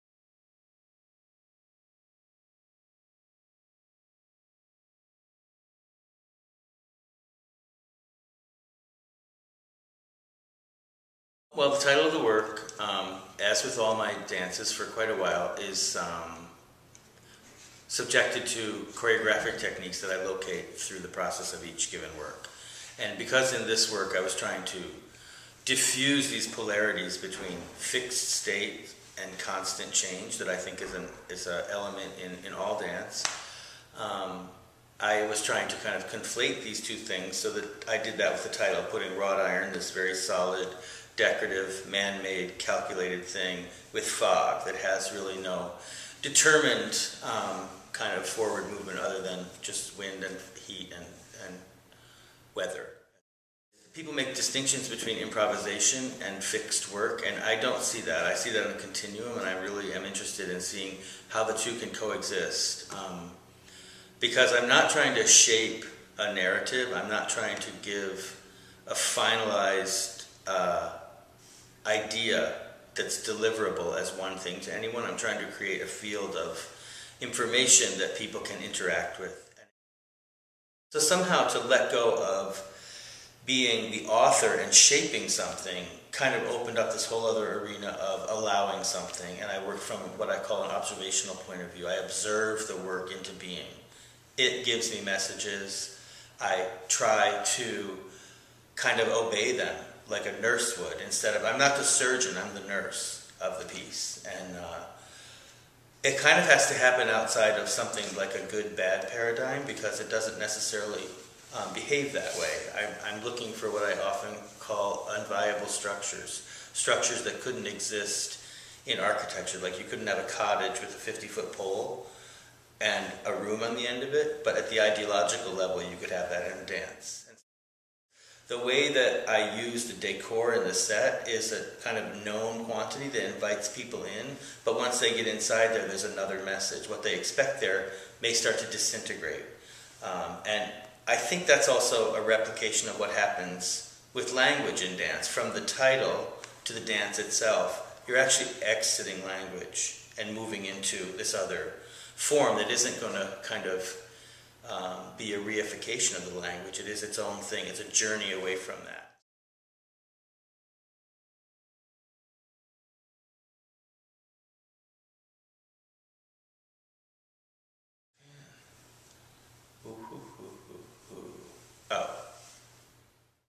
Tere O'Connor in conversation at REDCAT
Widely cited as one of America's most influential choreographic voices, Tere O'Connor is known as a master of composition and an evocative manipulator of gesture and theatricality. Here he sits down at REDCAT before dress rehearsal to discuss "Wrought Iron Fog."